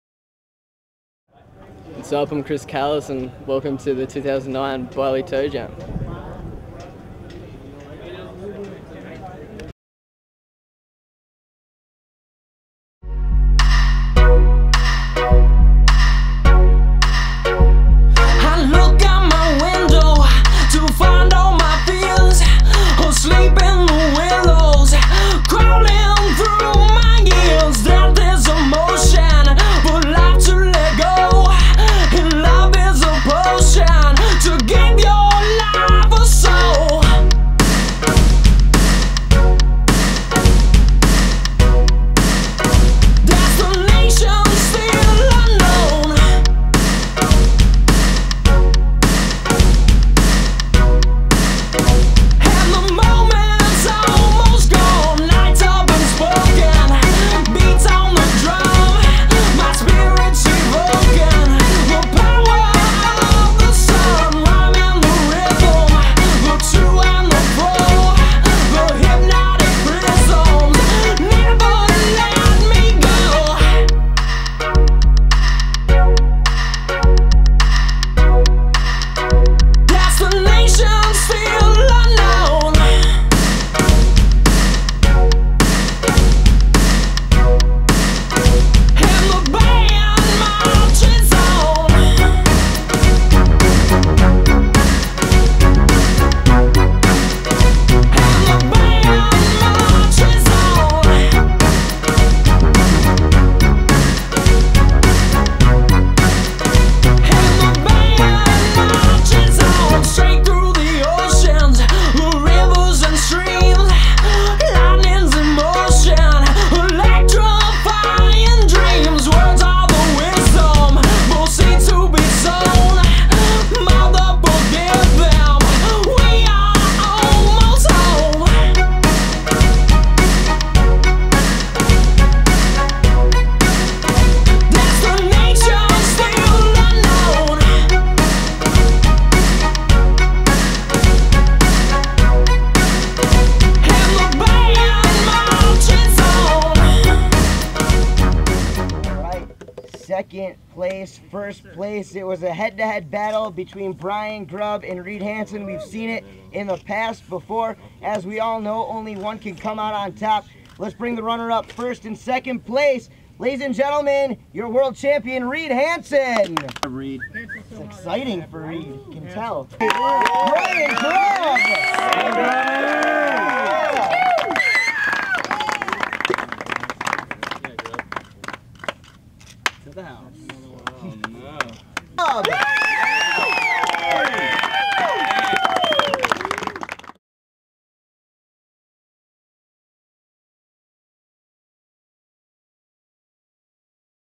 Highlights from the 2009 Byerly Toe Jam: Stop 3
Check out the action at the 2009 Byerly Toe Jam in Orlando Florida. Stop 3 of the 4 stop series. The top wake riders in the world came out to show their skills behind the fully weight Air Natique and Put on quite a show. Landing flawless runs and tricks only seen in videos before. Brian Grubb Pulled off a perfect Finals Run to beat out Reed Hansen. Check out the Full Write up and Pictures Exclusively on GrindTV.